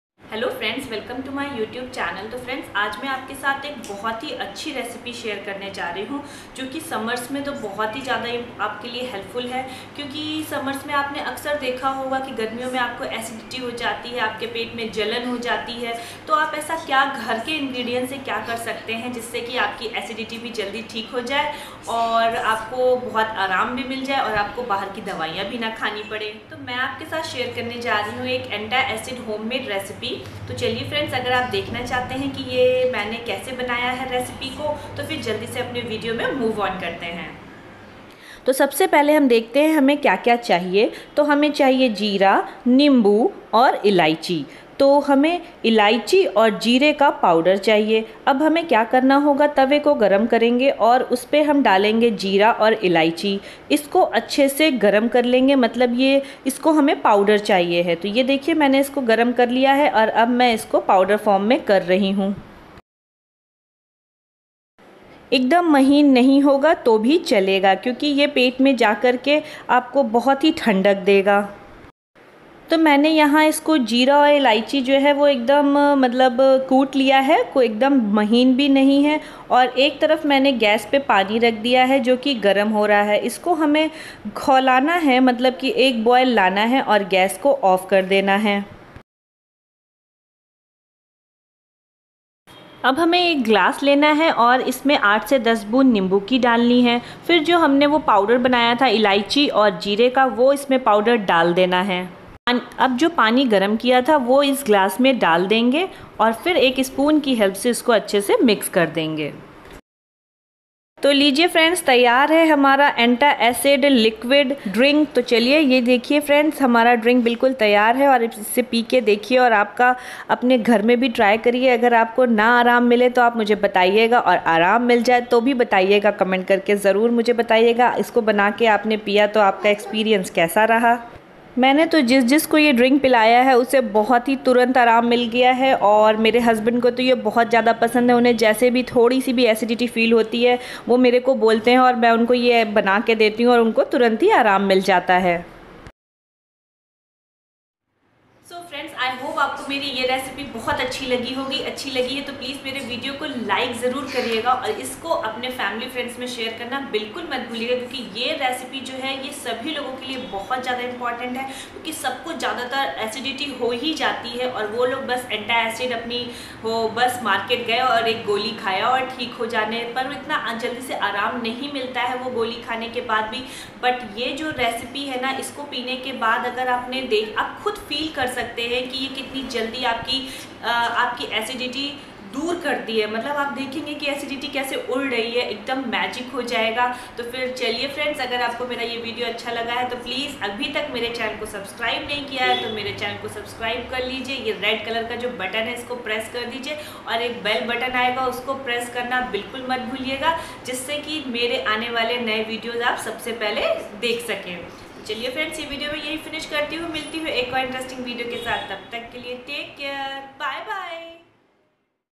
2 मिनट में एसिडिटी दूर करे।diy home made Antacid drink.indianyoutuberbulbul
hello friends

Home made Antacid drink.
Cumin seeds (jeera 1/2 spoon)
lemon (8 drop)
Cardamom (1)

please don't use my any video without my permission.

bye bye
take care 😊